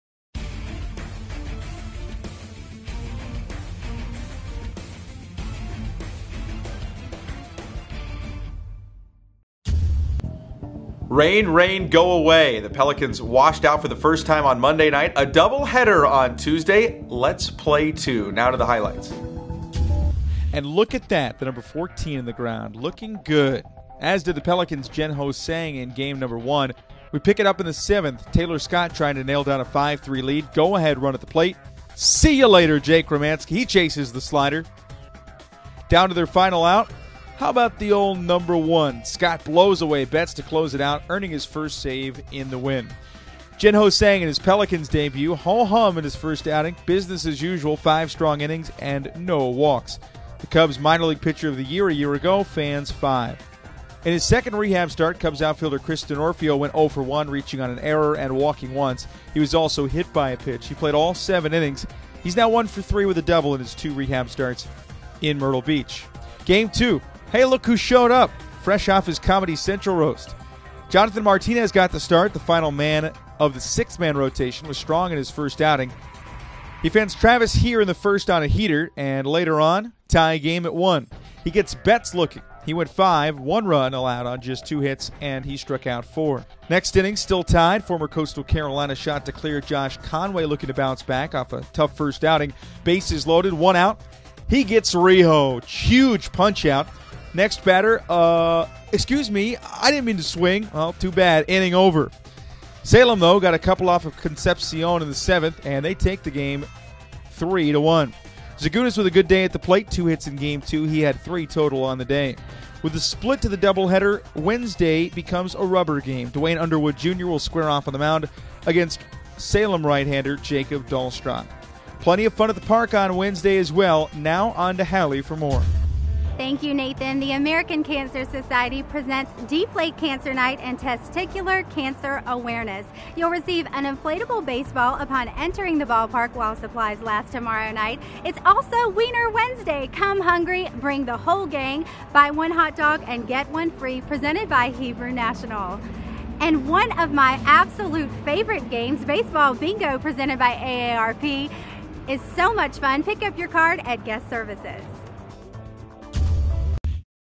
Pelicans Recap - April 14, 2015
The Myrtle Beach Pelicans, Class A-Advanced affiliate of the Chicago Cubs, split their first doubleheader of the 2015 season, taking a 5-3 victory over the Salem Red Sox before falling 3-1 in the nightcap at TicketReturn.Com Field at Pelicans Ballpark on Tuesday. Righthander Jen-Ho Tseng picked up the win in his Pelicans debut, and Cubs outfielder Chris Denorfia went 0-for-1 with a walk in his second start on a rehab assignment.